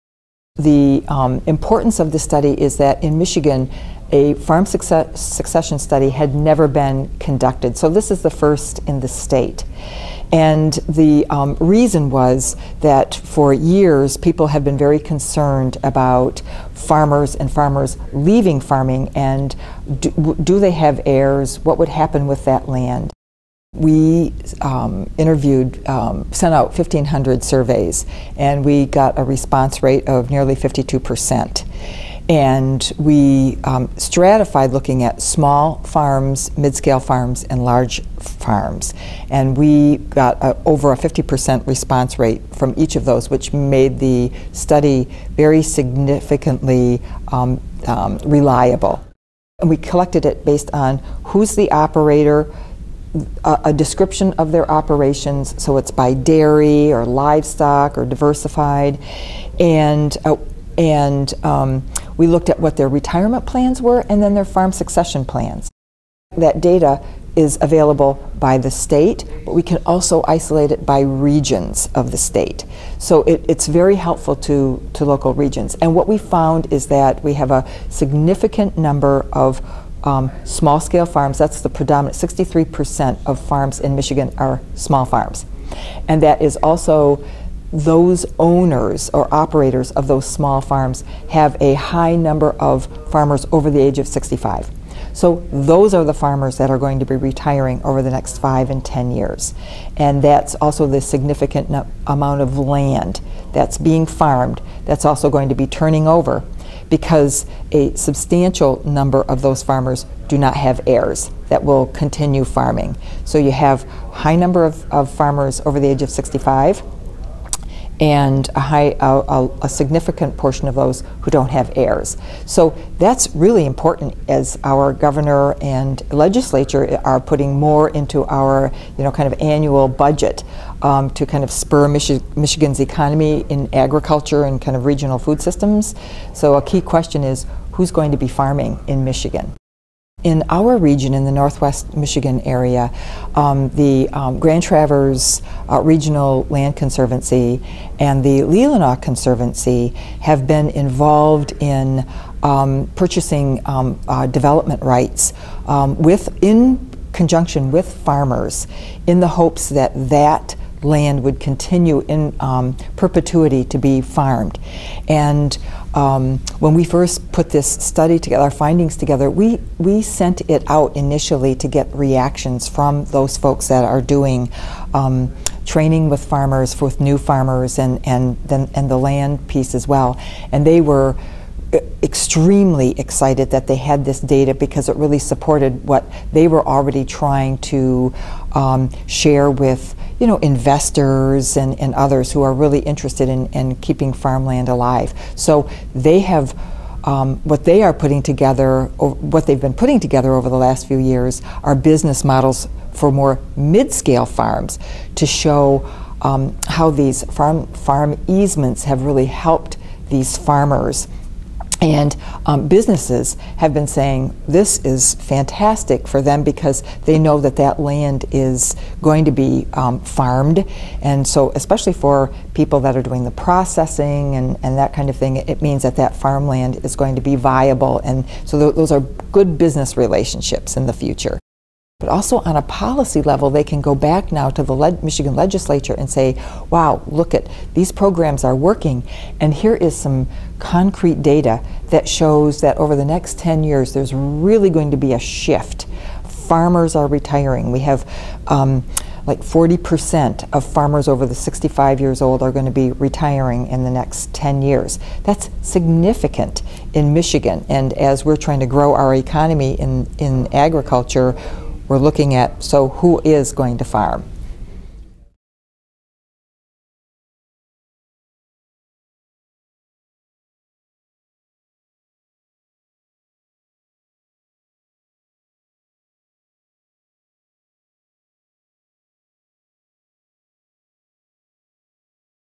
Farm Succession Study Looks at Future of Michigan Farmland
The results of a Michigan Farm Succession Study were presented at a meeting of the Northwest Michigan Agriculture and Food System Sector Alliance in Traverse City. Watch the video to find out what the study revealed about the future of Michigan farms.